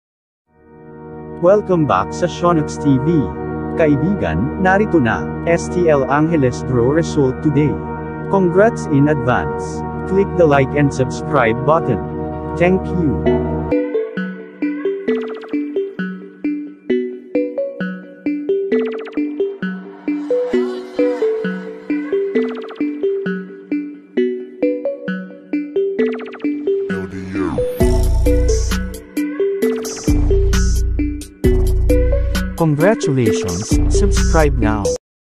STL Angeles result today 4pm draw afternoon result 2nd draw Philippines September 6, 2024 Friday
Today September 6, 2024 Friday
STL Small Town Lottery, Second draw result for STL ANGELES STL Pares Game.
STL ANGELES 2ND DRAW RESULT TODAY
GAME: STL ANGELES 2ND DRAW

HERE IS THE STL ANGELES RESULT TODAY (4:00 PM DRAW RESULT).